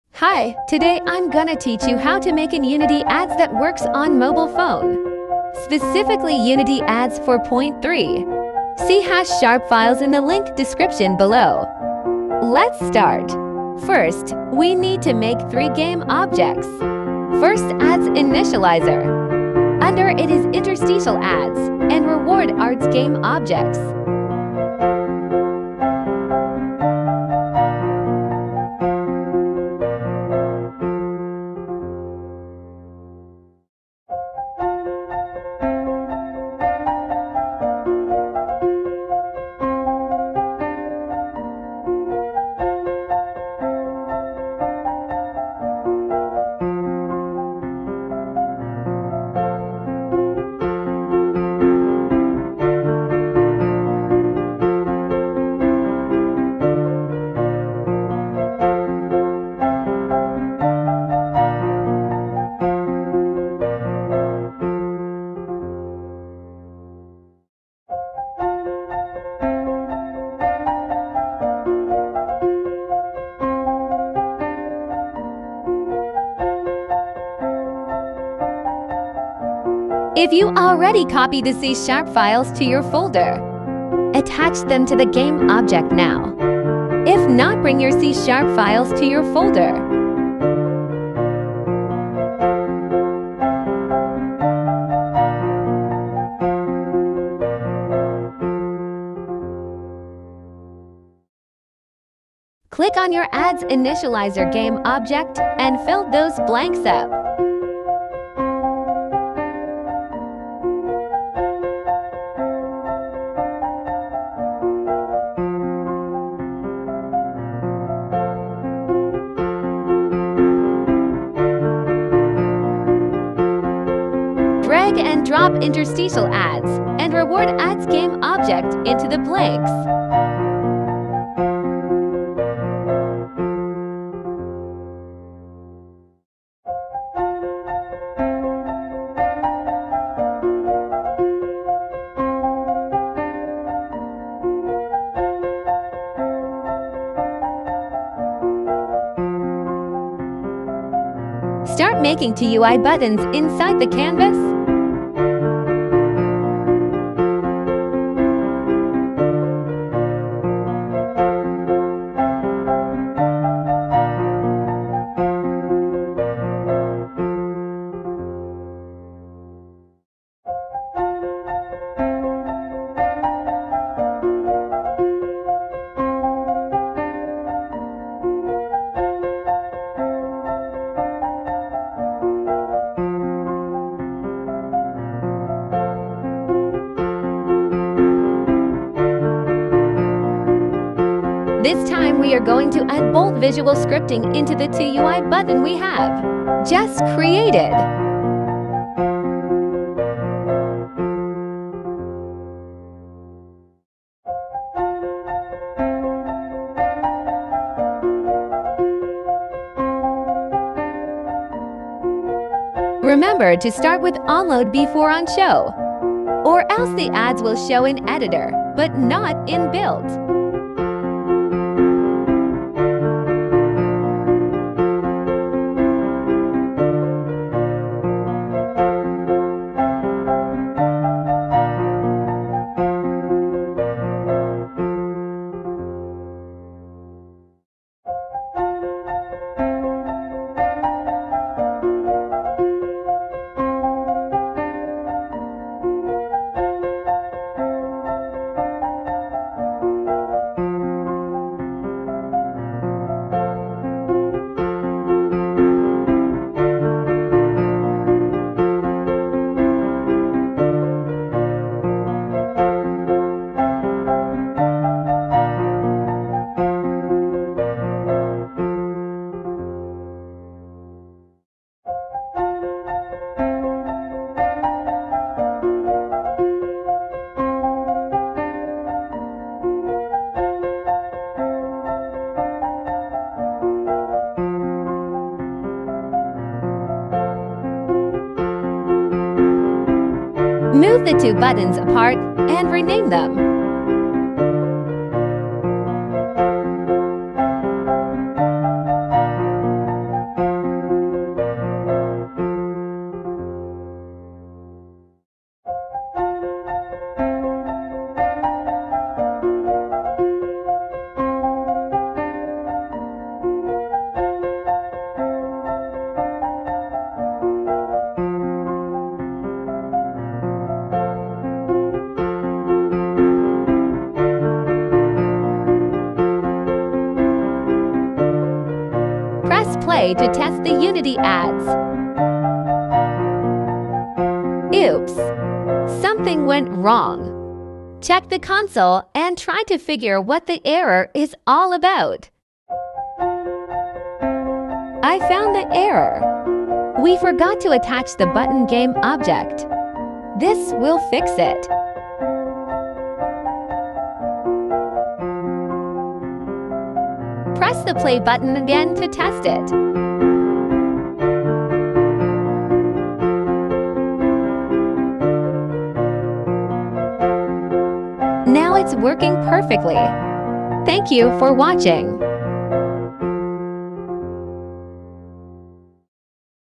Unity Ads not loading on built (Unity Ad 4.3 Bolt Visual Scripting)
Recent update of Unity Ads cause old scripts to not work correctly
This is how I solved it.